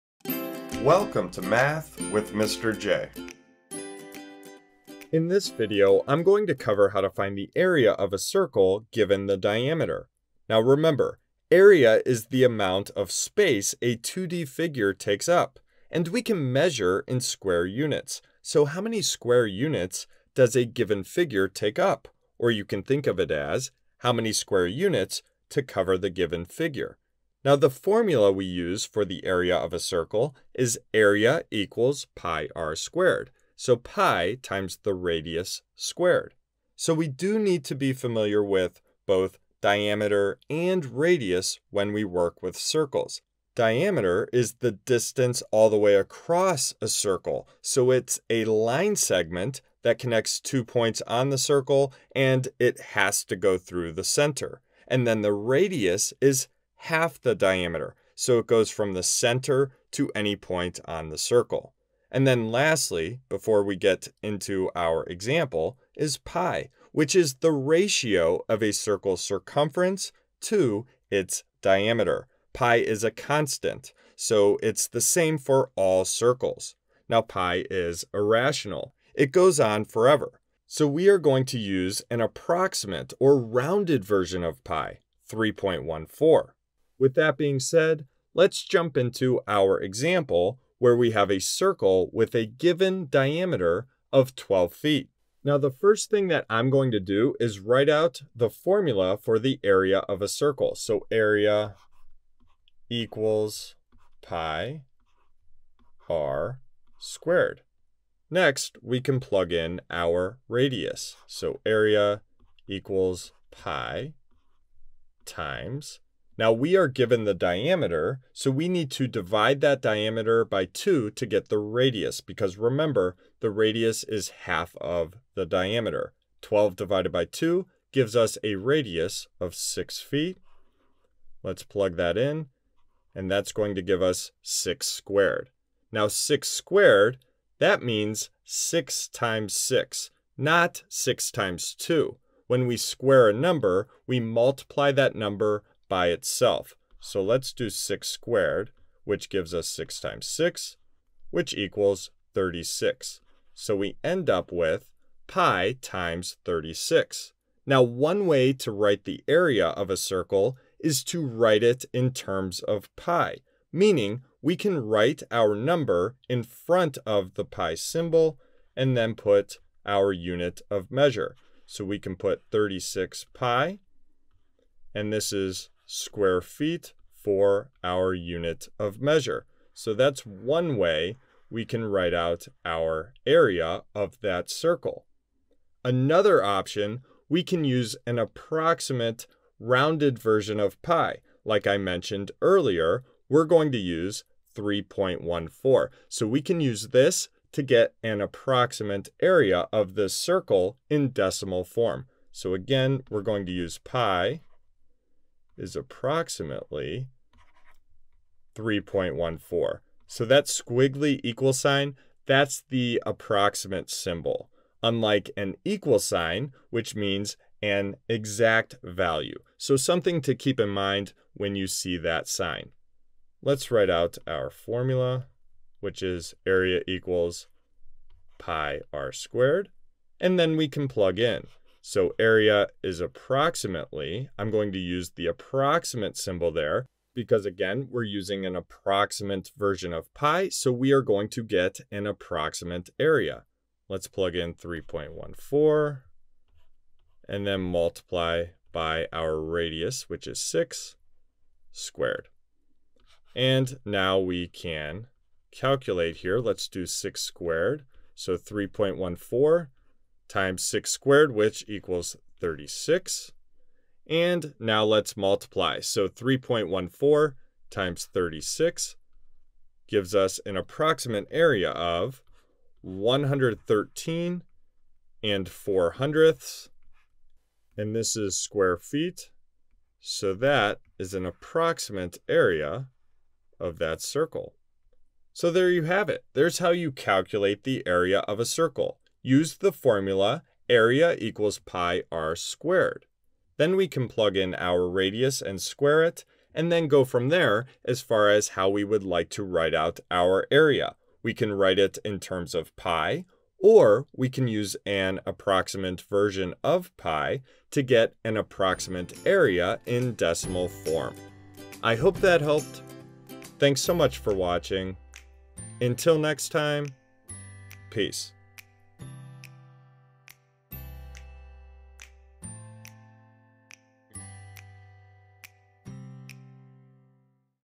How to Find the Area of a Circle Given the Diameter | Math with Mr. J
Welcome to How to Find the Area of a Circle Given the Diameter with Mr. J! Need help with how to calculate the area of a circle? You're in the right place!

Whether you're just starting out, or need a quick refresher, this is the video for you if you're looking for help with how to find the area of a circle given the diameter. Mr. J will go through an example of how to find the area of a circle and explain the steps of how to calculate the area given the diameter.

Hopefully this video is what you're looking for when it comes to finding the area of a circle given the diameter.